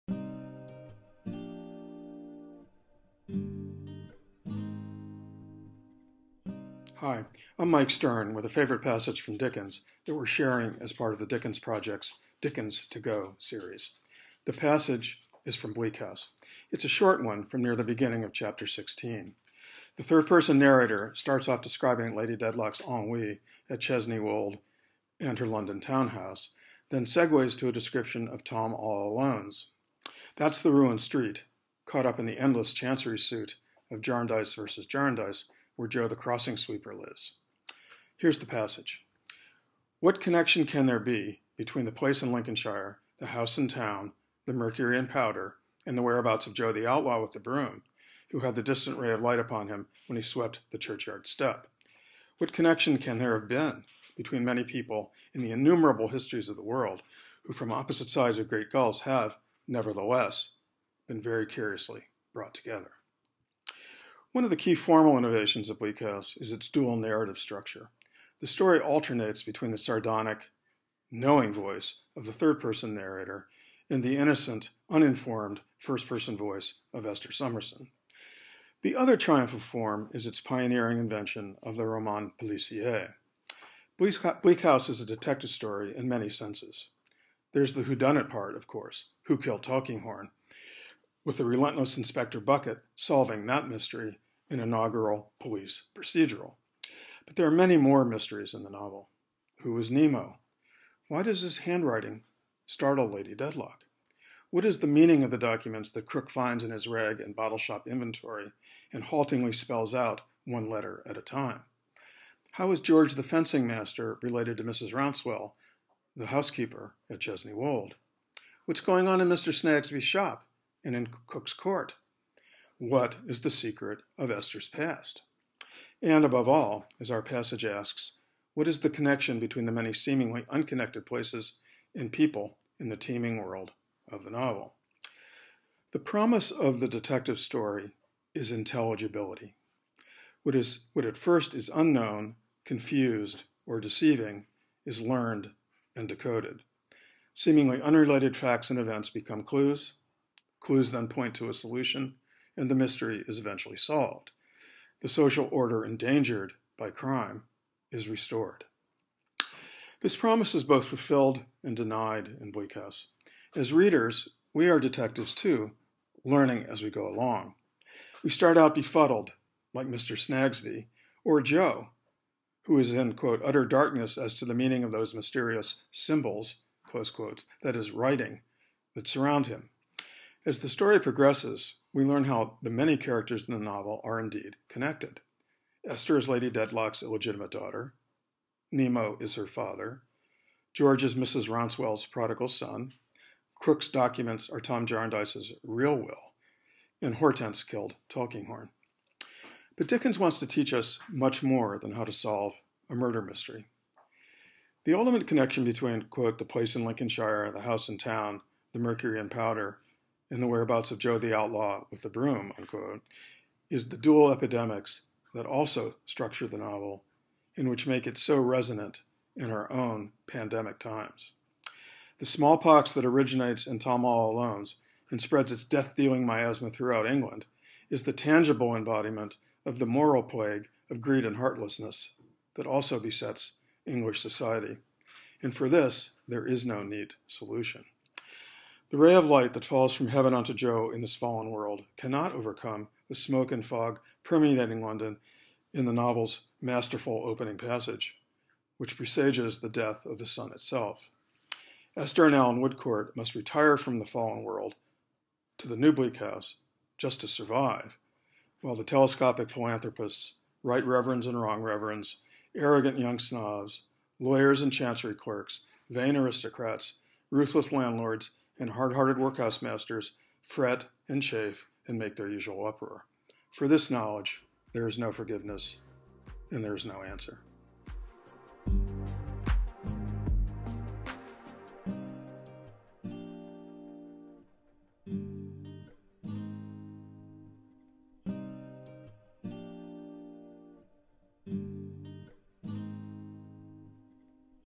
Dickens-to-Go: The Dickensian Reader as Detective
Friends of the Dickens Project Board Member and filmmaker, Michael Stern, discusses how the uncovering of connections between seemingly unconnected places and people in 'Bleak House' can help us to understand societal epidemics.